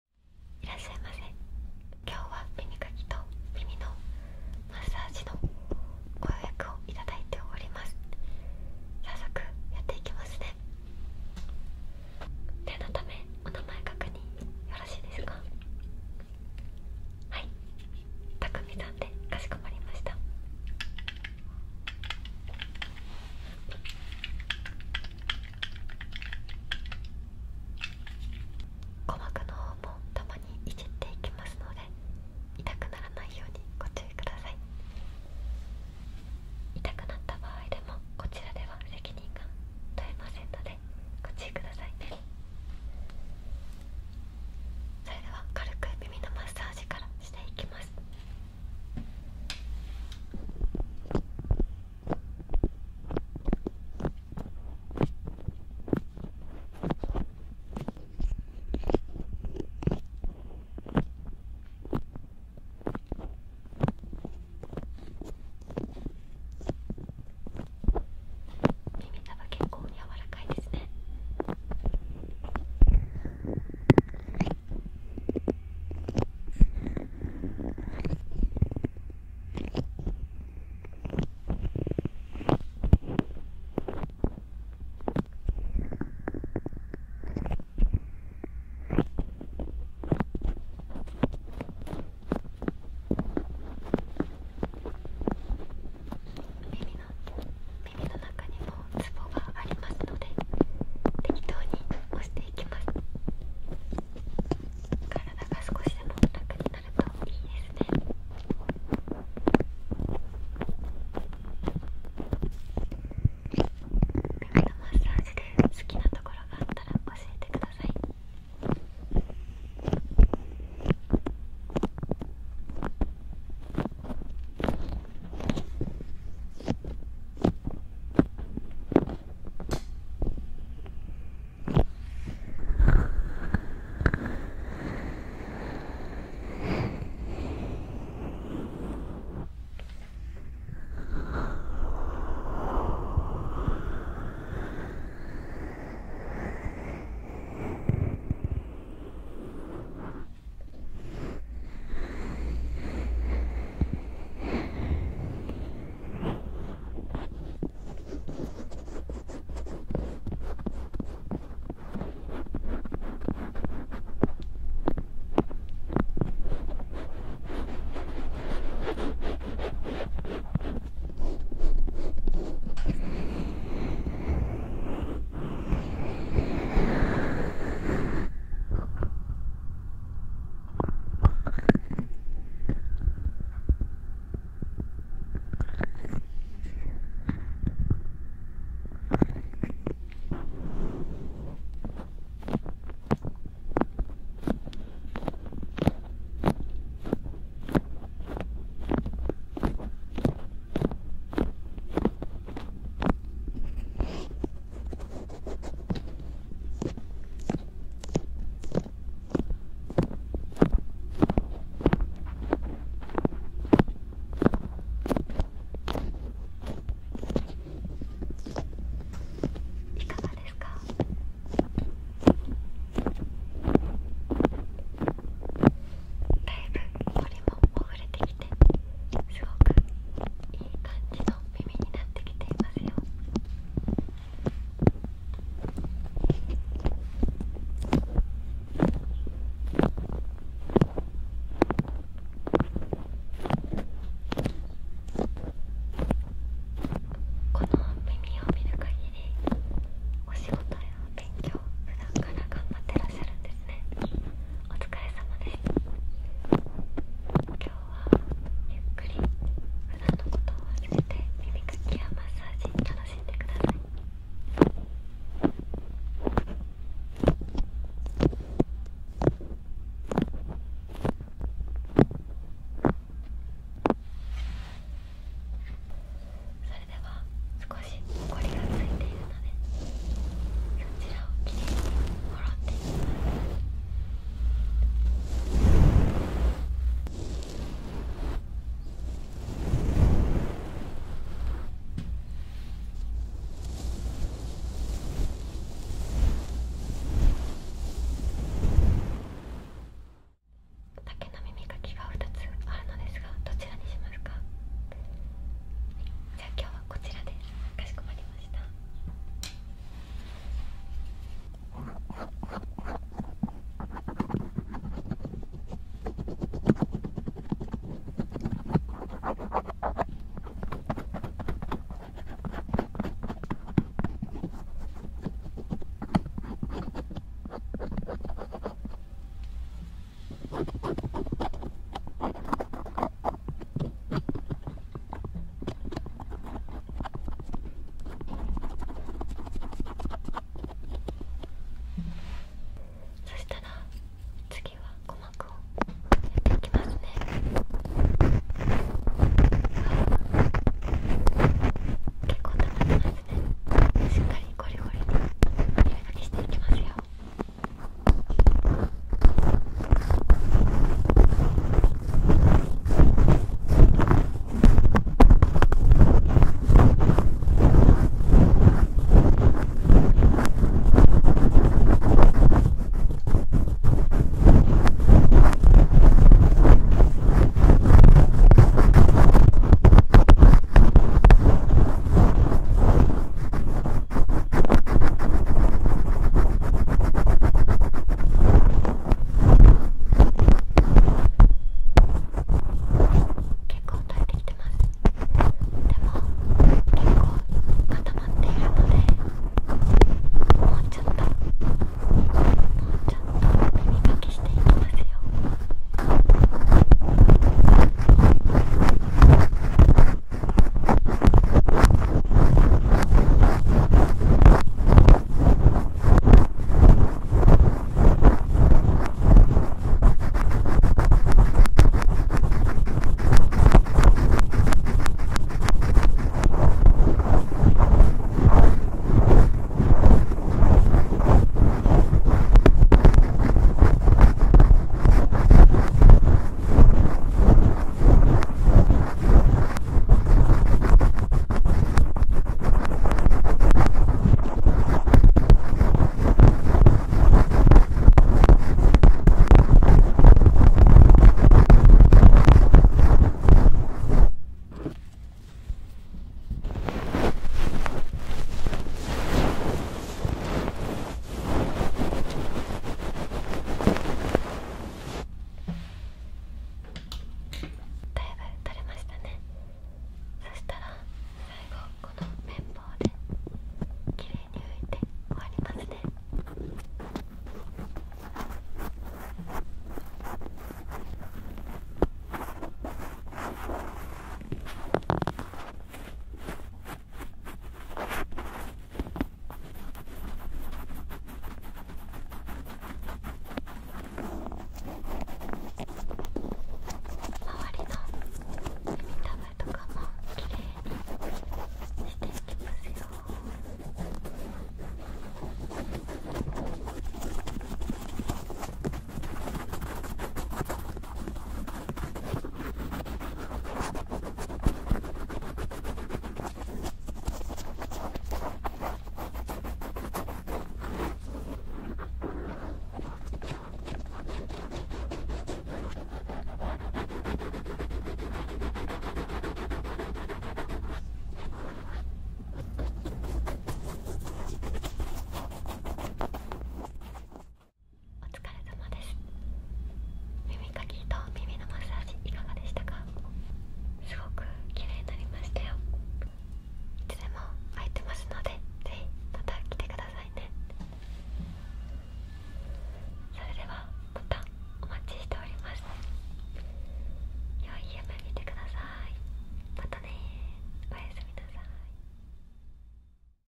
【ASMR】雑で眠れる耳かき屋さんロールプレイ（Rough earpicker and massage role play.)
こんばんは！
本日はJGA様とのコラボが叶いました✨
いつも応援してくださっている皆様に
本当に感謝しています！！

JGA様の動画でもUPされていますので
是非覗いてみてください😌